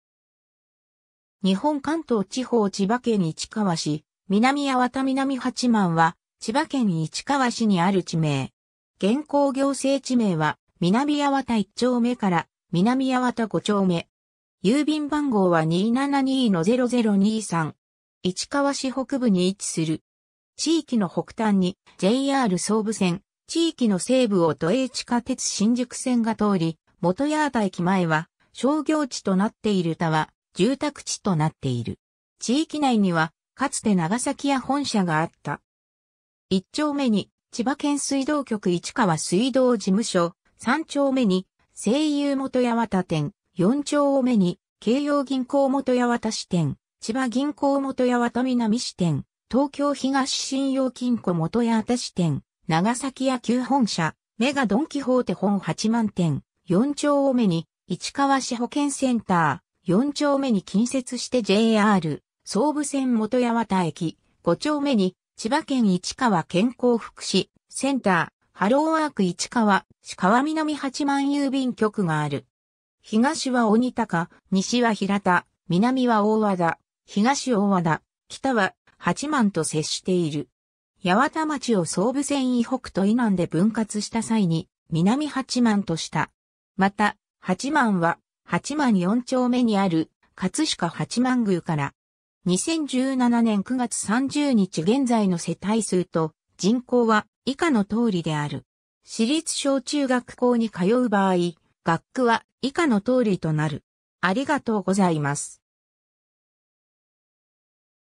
南八幡 (市川市)
市川市の町・字
日本 」 関東地方 」 千葉県 」 市川市 」 南八幡 (市川市) 南八幡（みなみやわた）は千葉県市川市にある地名。
現行行政地名は南八幡一丁目から南八幡五丁目。
 市川市北部に位置する。
地域の北端にJR総武線、地域の西部を都営地下鉄新宿線 が通り、本八幡駅前は商業地となっている他は、住宅地となっている。
地域内には、かつて長崎屋本社があった。
 一丁目に千葉県水道局市川水道事務所、三丁目に西友本八幡店、四丁目に京葉銀行本八幡支店、千葉銀行本八幡南支店、東京東信用金庫本八幡支店、長崎屋旧本社・MEGAドン・キホーテ本八幡店、四丁目に市川市保健センター、四丁目に近接してJR総武線本八幡駅、五丁目に千葉県市川健康福祉センター（市川保健所）、ハローワーク市川、市川南八幡郵便局がある。
 東は鬼高、西は平田、南は大和田、東大和田、北は八幡と接している。
 八幡町を総武線以北と以南で分割した際に「南八幡」とした。
また「八幡」は、八幡四丁目にある「葛飾八幡宮」から。
 2017年（平成29年）9月30日現在の世帯数と人口は以下の通りである。